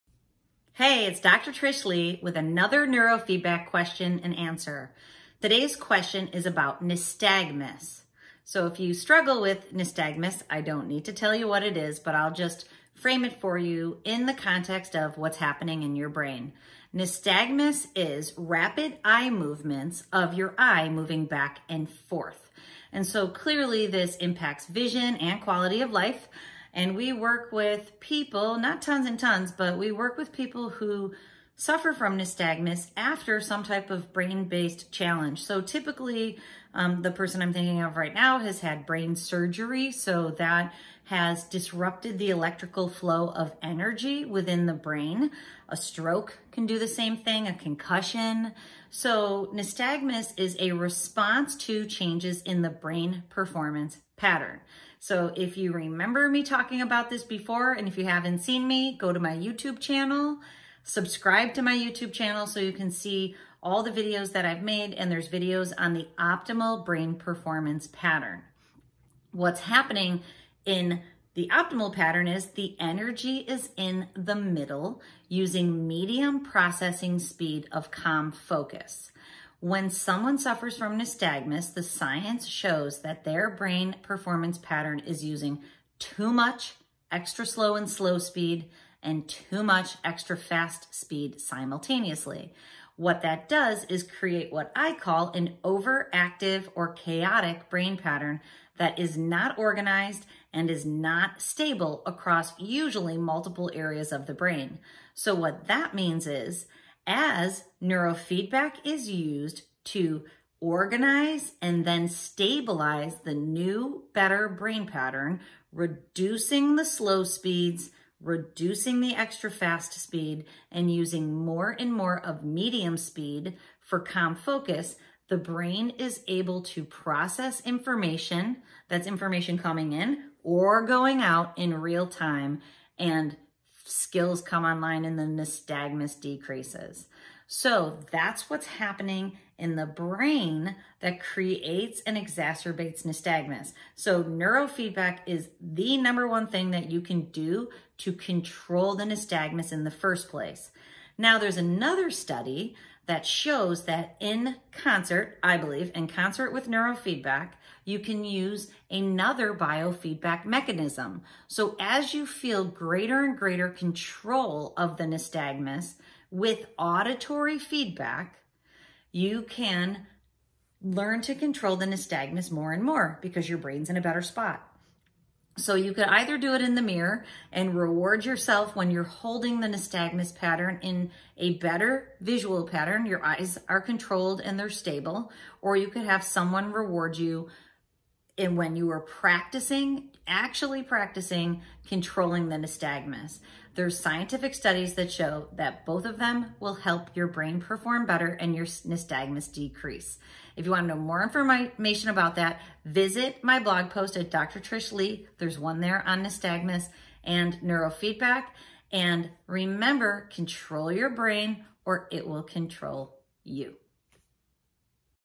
Neurofeedback To Improve Nystagmus
Dr. Trish Leigh tells you how Neurofeedback can help to improve nystagmus that is negatively impacting your life. And a regular brain pattern is causing your nystagmus to be worse. Find out out in this video.